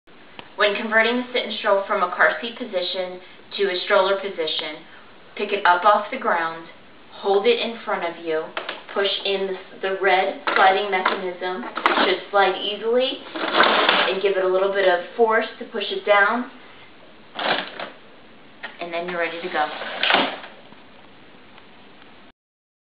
Switch Between Car Seat to a Stroller Steps LillyGold.com Sit & Stroll, 5 in 1 Carseat
LillyGold.com Sit & Stroll

lillygold.com, The Sit ‘n’ Stroll Convertible Car Seat to Baby Stroller
When you travel with a baby or toddler, domestic or internationally traveling light is usually not an option. The Sit ‘n’ Stroll convertible baby stroller by Lilly Gold, LLC is an international car seat, a stroller, and a dining booster seat, it can also be used when traveling by air as an FAA flight seat. The Sit ‘n’ Stroll is a stroller and car seat in one product that really does make life easier for you and more relaxing for your child. Safe, comfortable, well designed, and stylish, The Sit ‘n’ Stroll convertible car seat is truly innovative and clever. The Sit ‘n’ Stroll is suitable for babies from newborn to children up to 40 pounds, and converts easily from car seat to stroller and back again.